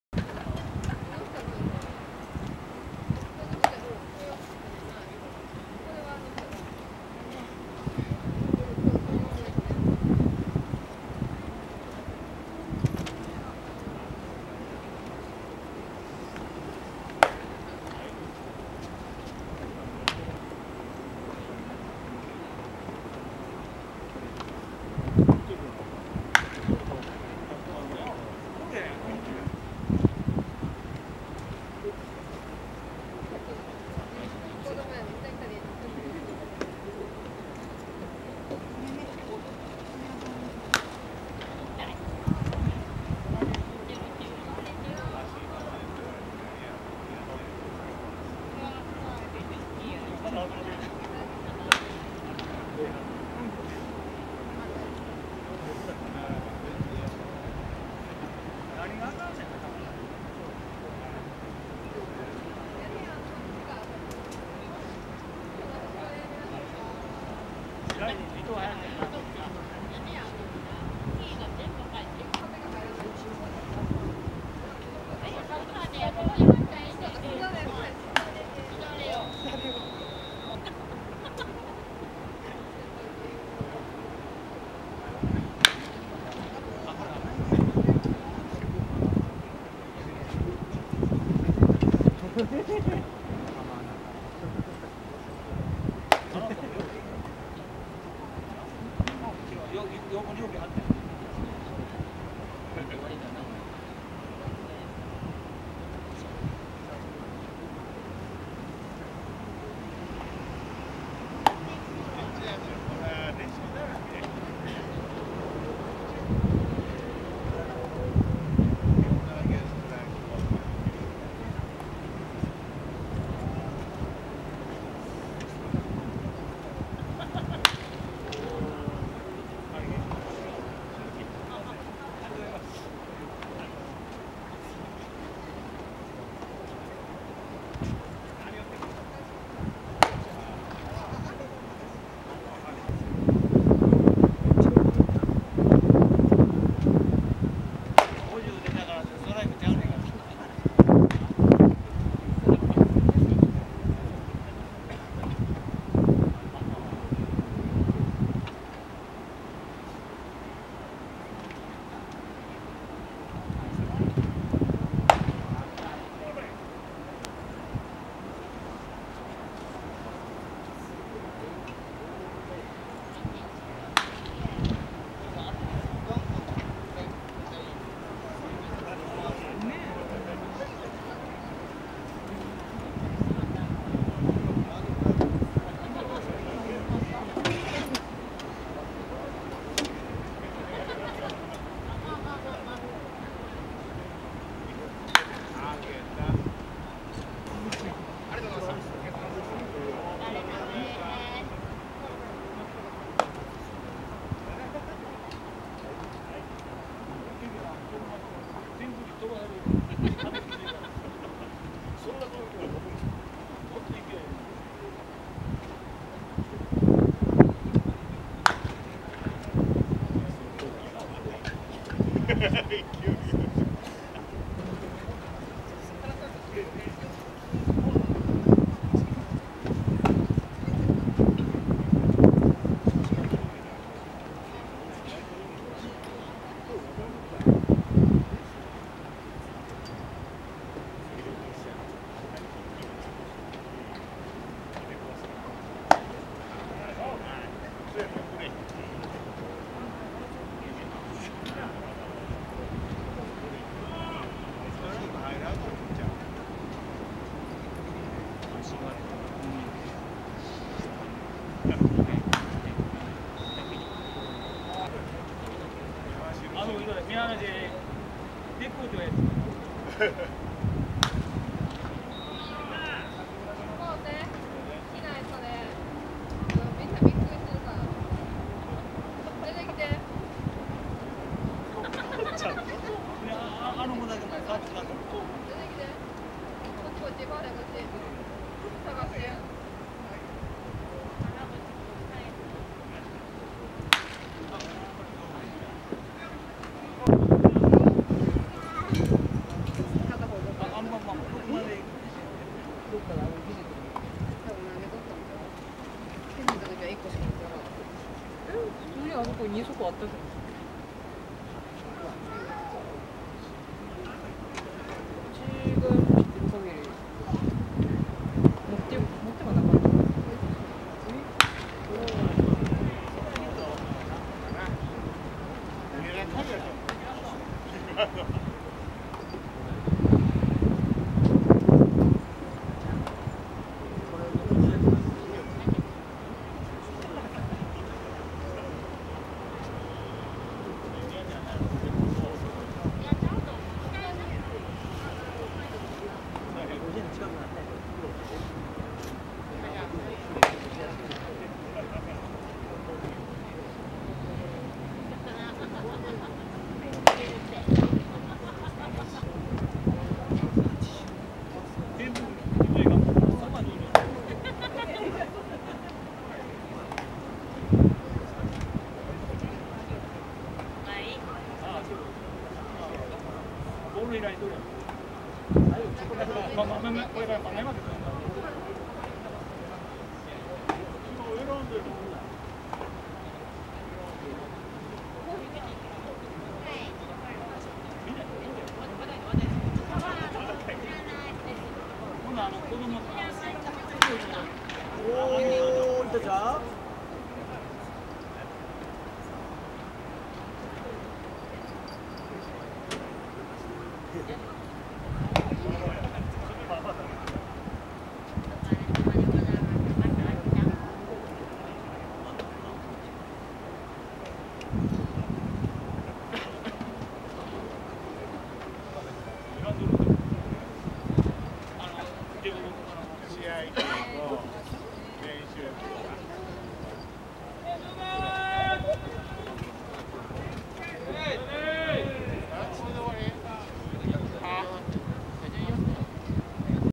【ドラ6新人】 2018.7.9 阪神タイガース 牧丈一郎投手（啓新高校） 【146km/h シート打撃に登板】
試合直後、熊谷、植田、高山選手らと対戦。